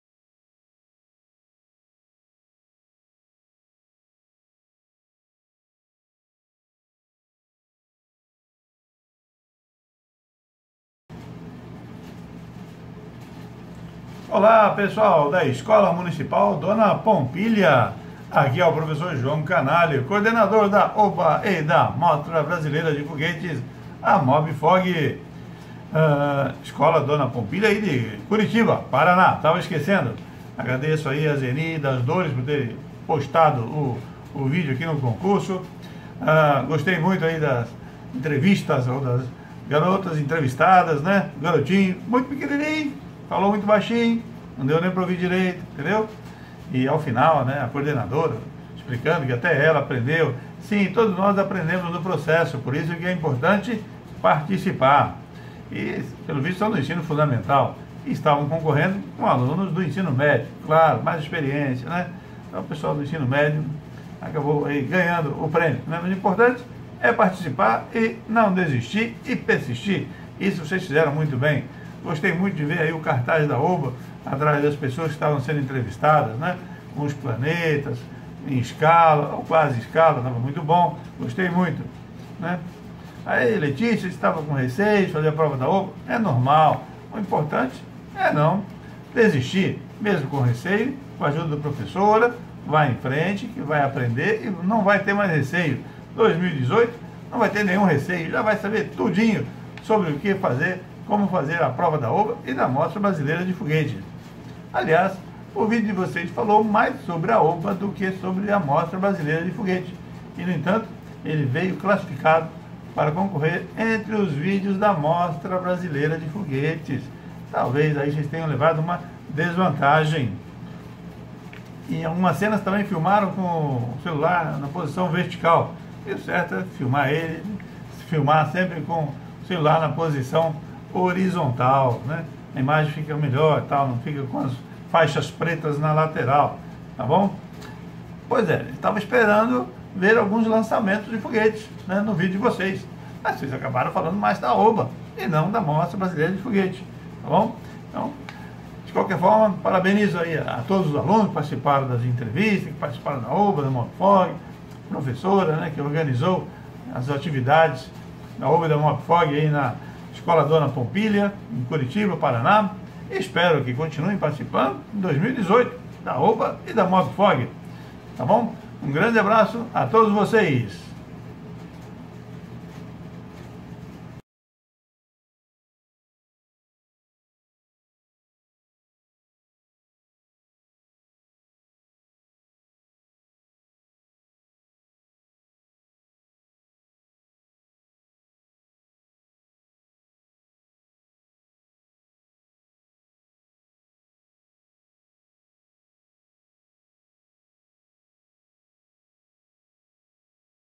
VÍDEO 05 - Escola Municipal Dona Pompilia - Curitiba, PR
Este é o vídeo de agradecimento à Escola Municipal Dona Pompilia, de Curitiba, PR, por ter participado do concurso de vídeos da MOBFOG em 2017.